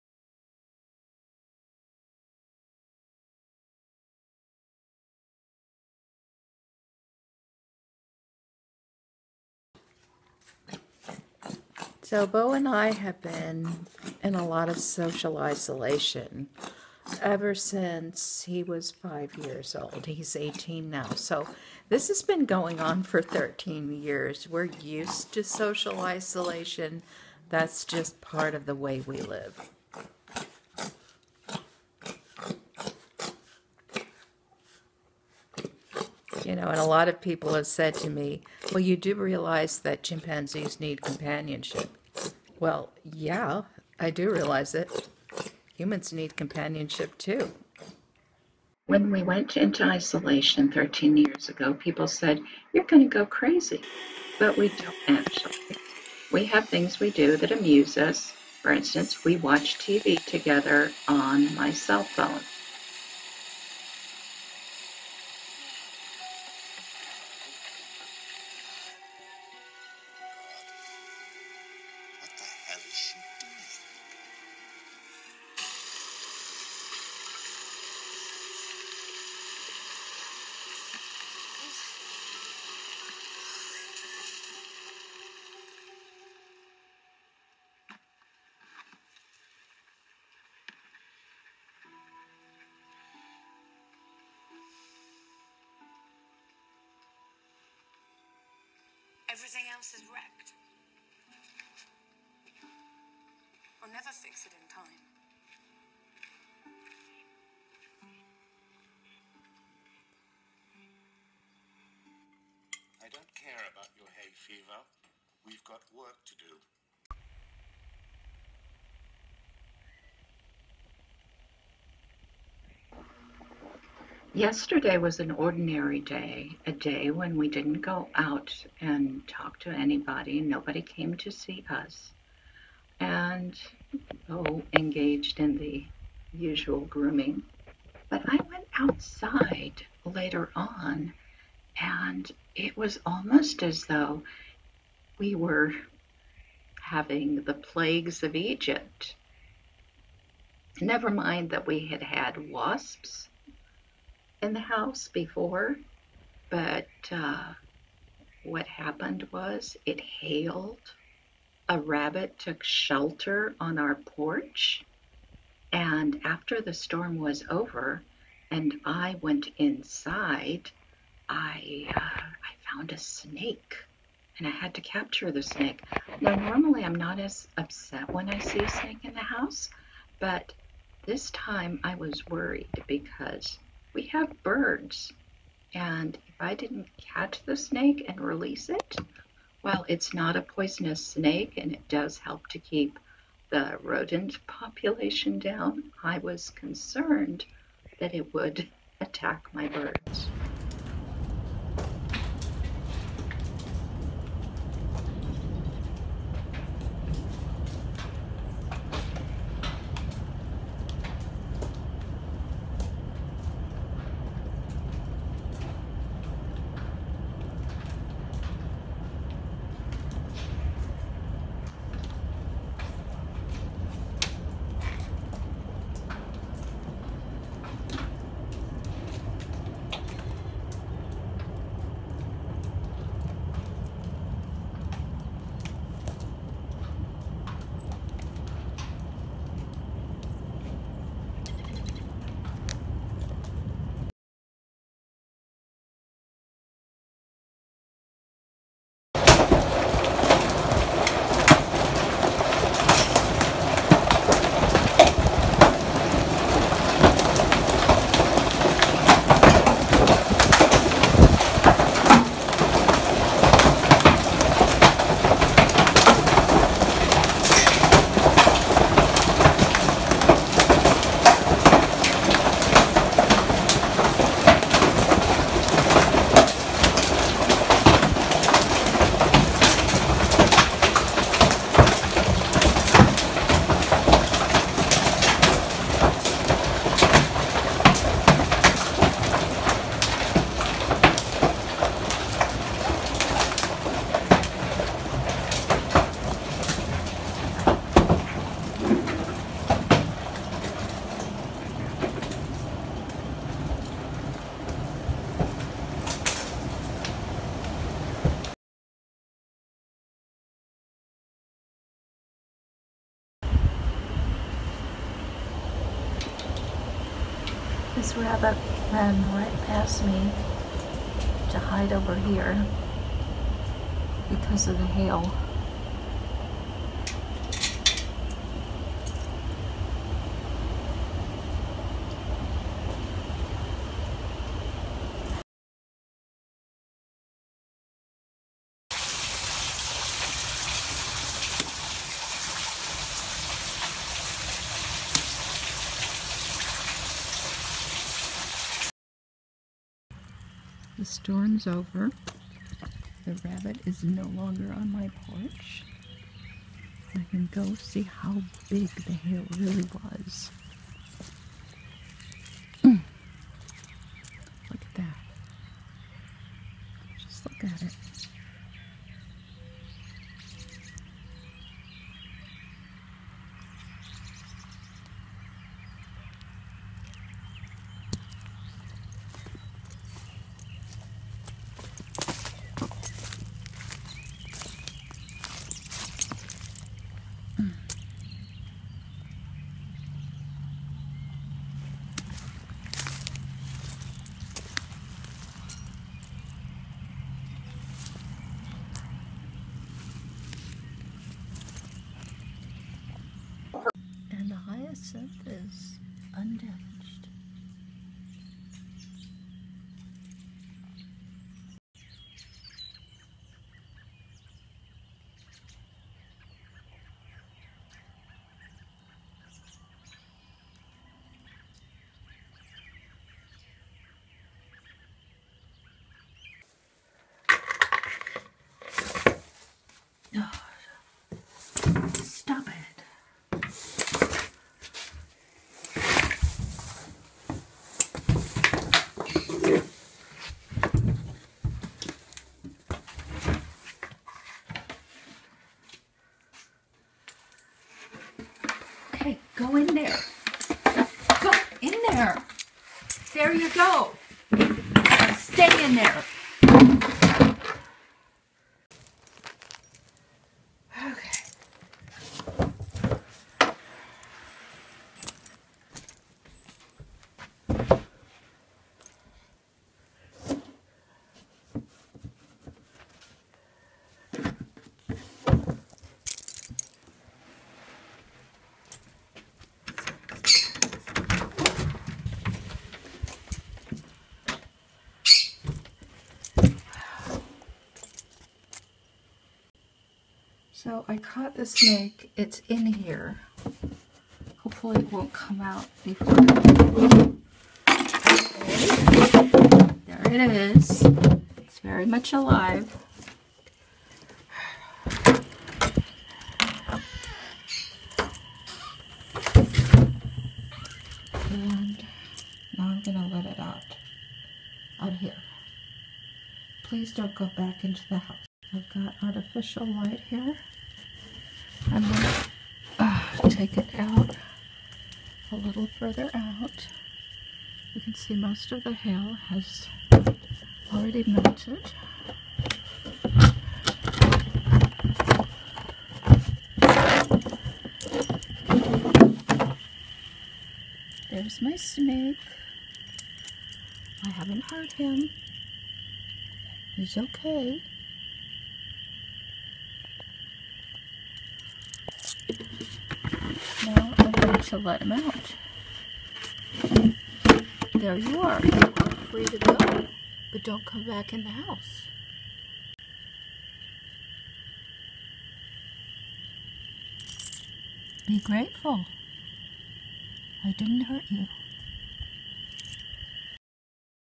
Bow and I Keep Smiling through the Crisis
We are in social isolation, which we are used to. But yesterday, it felt as if we were going through the plagues of Egypt. First there was hail the size of golf balls, and later a large black rat snake was in the house. Nothing like real world problems to keep my mind off the virus.